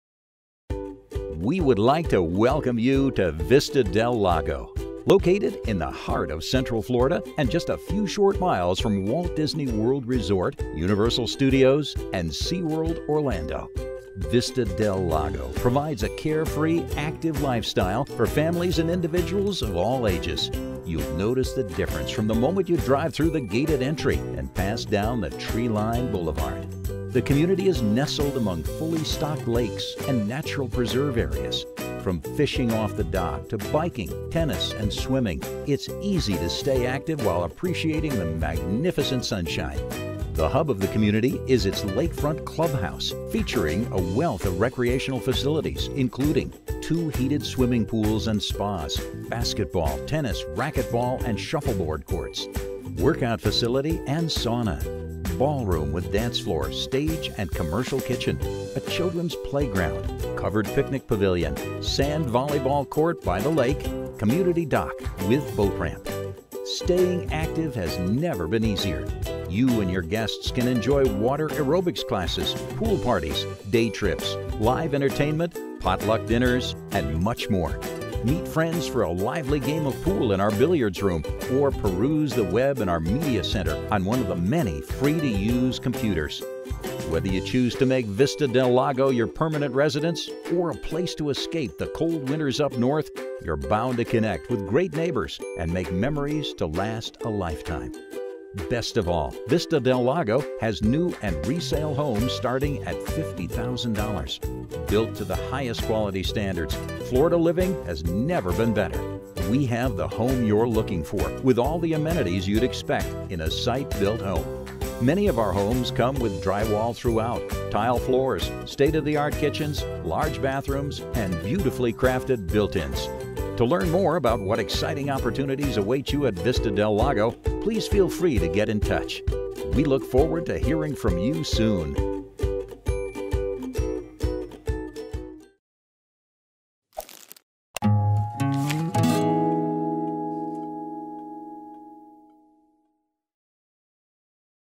Vista del Lago Community Video
Vista del Lago is an all-age, leased land, manufactured (mobile) home community set on the shores of Raccoon Lake, just 5 miles from Walt Disney World in beautiful Central Florida.